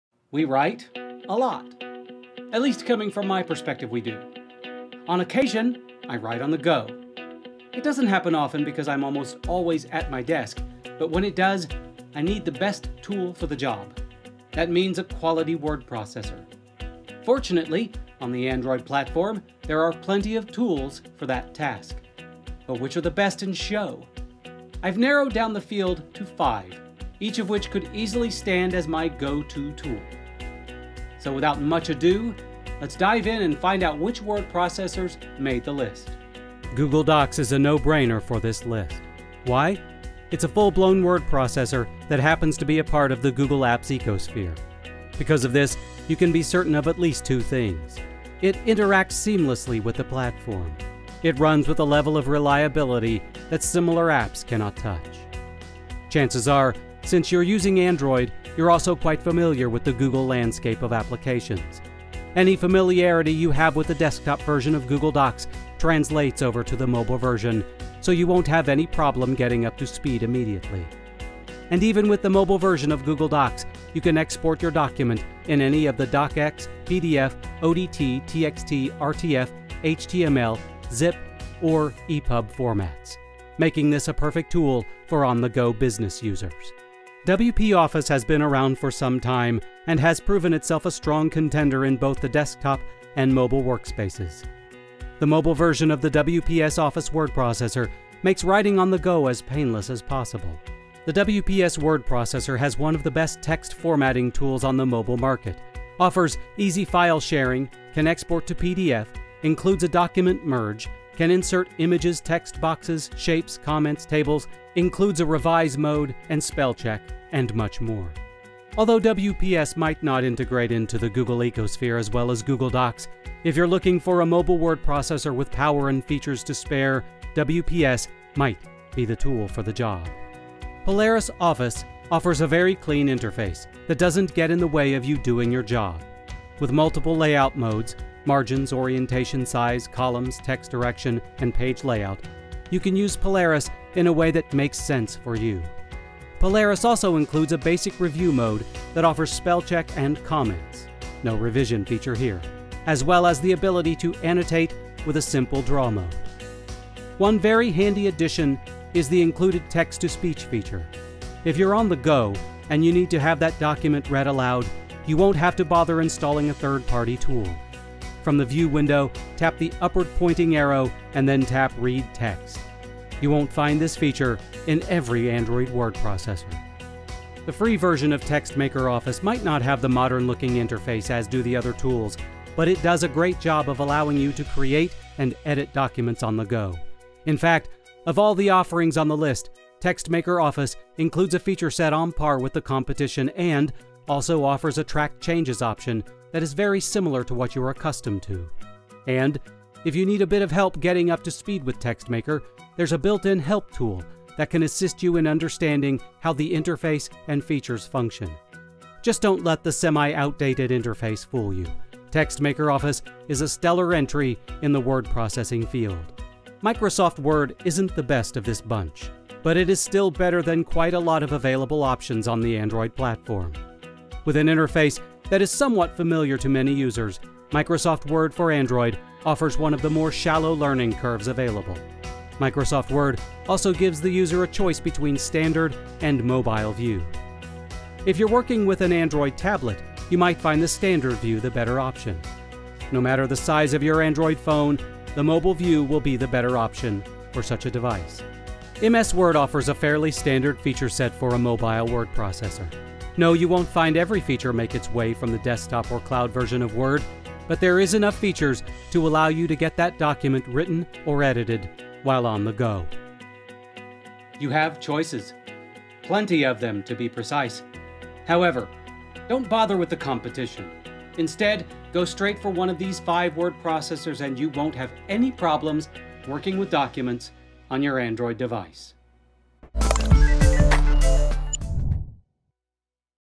Five best word processors for Android
If you need a word process for your Android device, Jack Wallen says you cannot go wrong with one of these five.